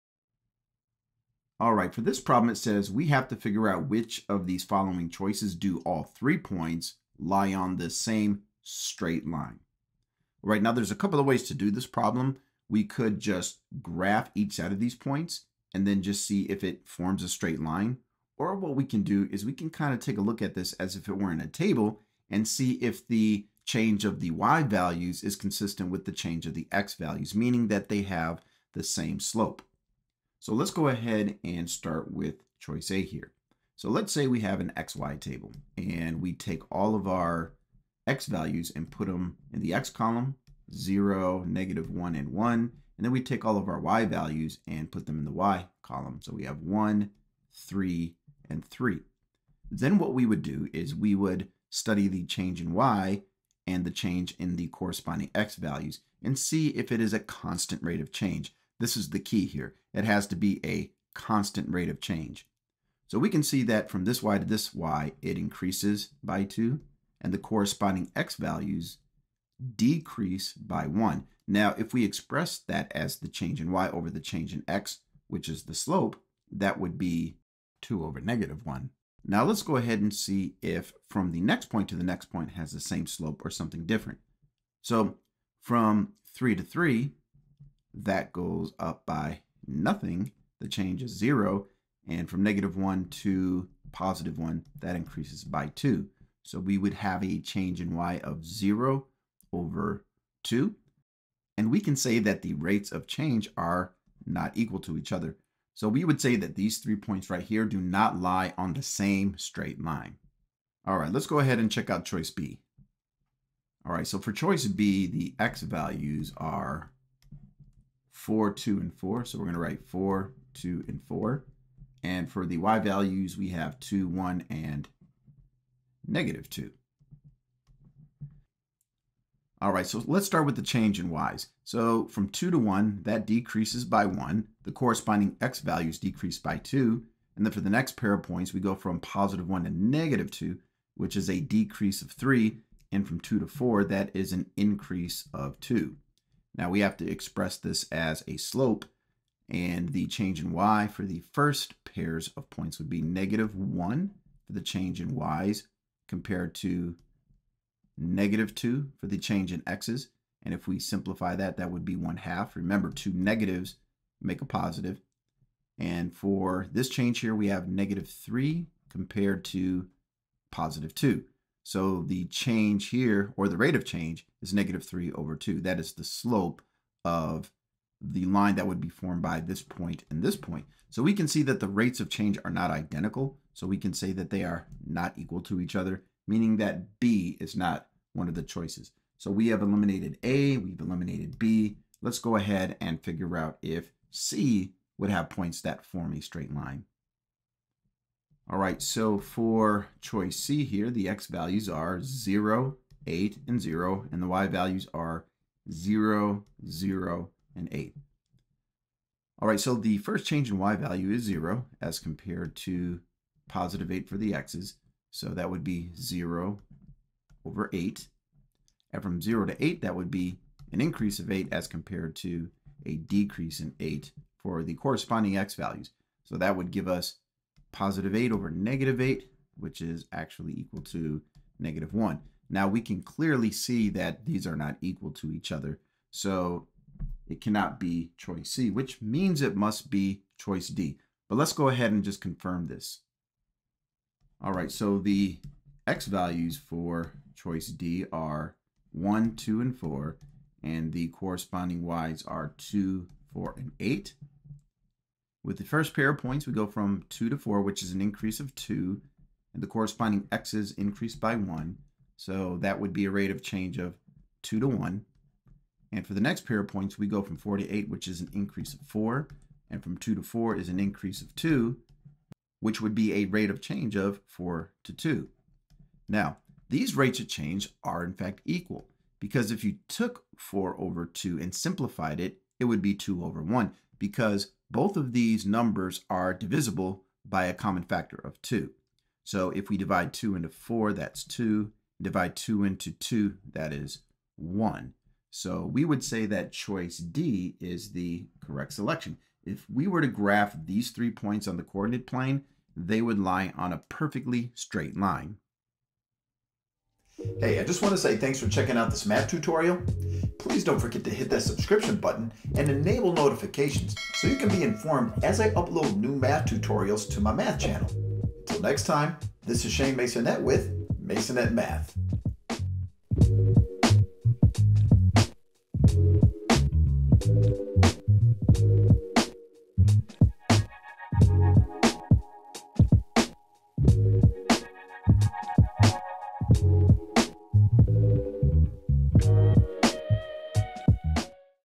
Do The Set Of Points Form A Line?
This video examines linear concepts.  Students must determine which set of given point would lie on a straight line.  Examine the relationship by determining if a constant rate of change exists between the sets of points.